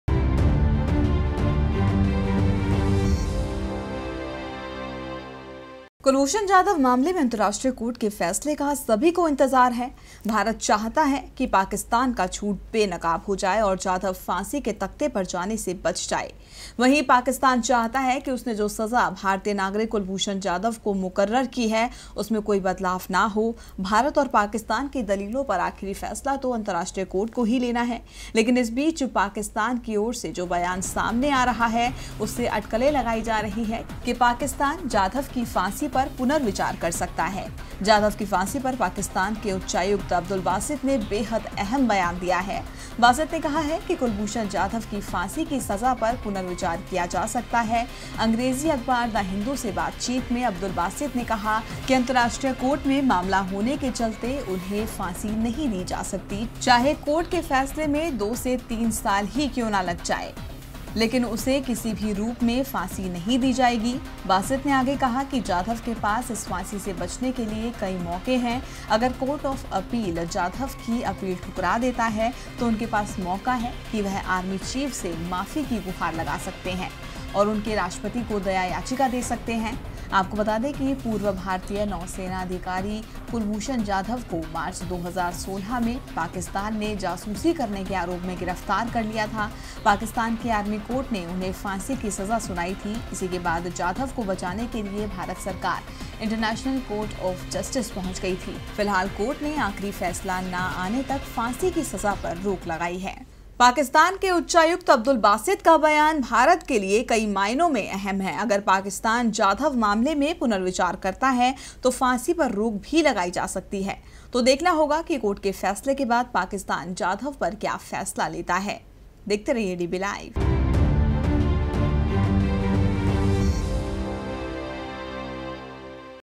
कुलभूषण जाधव की फांसी पर लग सकती है रोक |Abdul Basit says may be reconsideration on Kulbhushan
कुलभूषण जाधव मामले में अंतरराष्ट्रीय कोर्ट के फैसले का सभी को इंतजार है...भारत चाहता है कि पाकिस्तान का झूठ बेनकाब हो जाए और जाधव फांसी के तख्ते पर जाने से बच जाएं...वहीं पाकिस्तान चाहता है कि उसने जो सजा भारतीय नागरिक कुलभूषण को मुकर्रर की है उसमें कोई बदलाव ना हो...भारत और पाकिस्तान की दलीलों पर आखिरी फैसला तो अंतरराष्ट्रीय कोर्ट को ही लेना है...लेकिन इस बीच पाकिस्तान की ओर से जो बयान सामने आ रहा है उससे अटकलें लगाई जा रही हैं कि...पाकिस्तान जाधव की फांसी पर पुनर्विचार कर सकता है...जाधव की फांसी पर पाकिस्तान के उच्चायुक्त अब्दुल बासित ने बेहद अहम बयान दिया है। बासित ने कहा कि...कुलभूषण यादव की फांसी की सजा पर पुनर्विचार किया जा सकता है। अंग्रेजी अखबार द हिंदू से बातचीत में अब्दुल बासित ने कहा कि...अंतर्राष्ट्रीय कोर्ट में मामले होने के चलते उन्हें फांसी नहीं दी जा सकती चाहे कोर्ट के फैसले में दो से तीन साल ही क्यों ना लग जाए, लेकिन उसे किसी भी रूप से फांसी नहीं दी जाएगी। बासित ने आगे कहा कि जाधव के पास इस फांसी से बचने के लिए कई मौके हैं। अगर 'कोर्ट ऑफ अपील' जाधव की अपील ठुकरा देता है तो उनके पास मौका है कि वह आर्मी चीफ से माफी की गुहार लगा सकते हैं और उसके राष्ट्रपति को दया याचिका दे सकते हैं। आपको बता दें कि...पूर्व भारतीय नौसेना अधिकारी कुलभूषण जाधव को मार्च 2016 में पाकिस्तान ने जासूसी करने के आरोप में गिरफ्तार किया था। पाकिस्तान की आर्मी कोर्ट ने उन्हें फांसी की सजा सुनाई थी। इसी के बाद जाधव को बचाने के लिए भारत सरकार इंटरनेशनल कोर्ट ऑफ जस्टिस पहुंच गई थी। फिलहाल कोर्ट ने आखिरी फैसला ना आने तक फांसी की सजा पर रोक लगाई है.